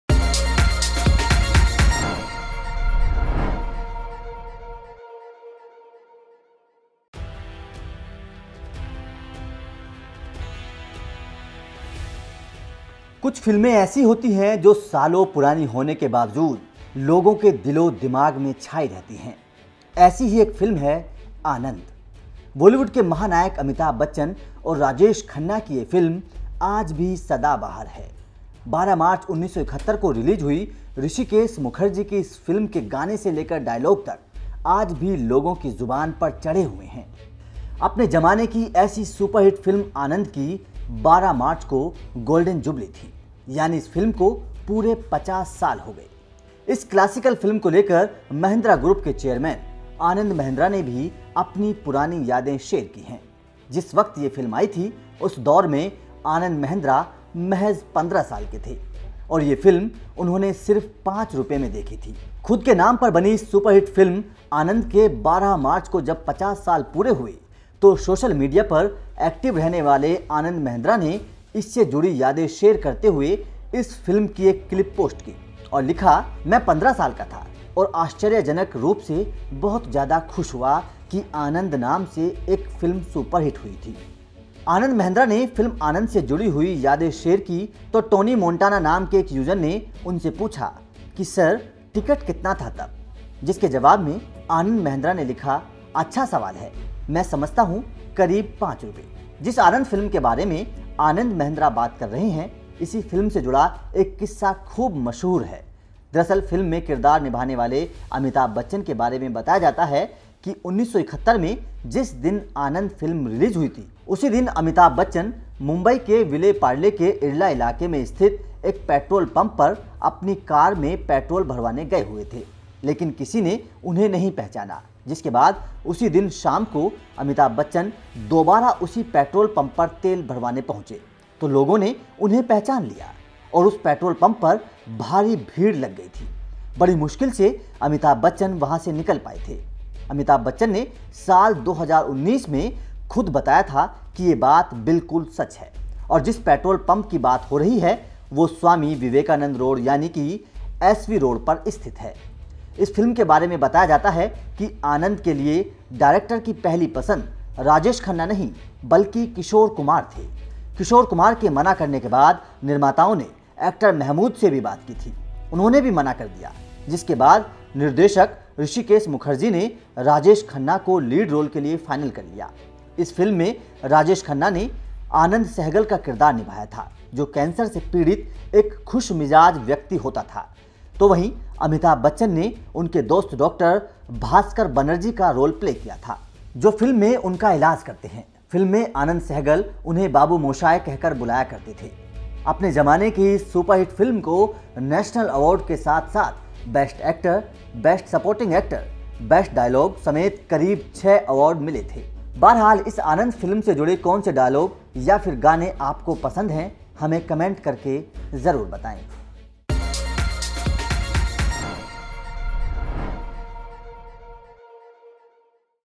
जानिये Anand Mahindra ने 15 साल में देखी थी कौन सी फिल्म ?
जानिए वो कौन सी फिल्म है जिसे महिंद्रा ग्रुप के चेयरमैन आनंद महिंद्रा ने सिर्फ पांच रुपये में देखी थी… और आज भी ये फिल्म सुपरहिट है !

rishabh
dheerendra